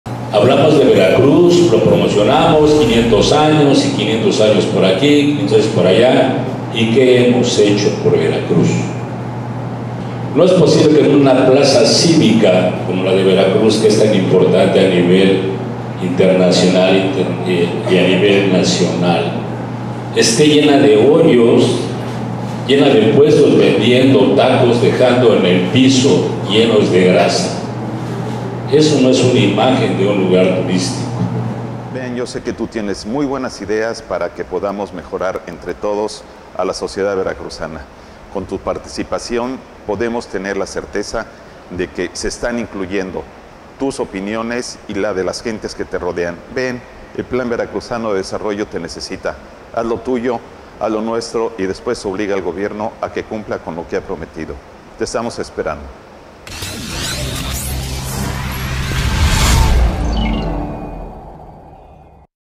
Con pésimas carreteras no hay turismo en Veracruz, levantan la voz los empresarios turísticos
Con propuestas de la ciudadanía se realizará el PLAN VERACRUZANO DE DESARROLLO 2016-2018 informo el Coordinador Estatal, Francisco Montfort Guillén.
Destaco que más de mil propuestas se han recibido cunado aun faltan por realizarse los Foros de la región del Cordoba-Orizaba los días 7 y 8 de Septiembre y la zona Coatzacolcos-Minatitlan los días 12 y 13 de Septiembre.
Todavía estas a tiempo de participar.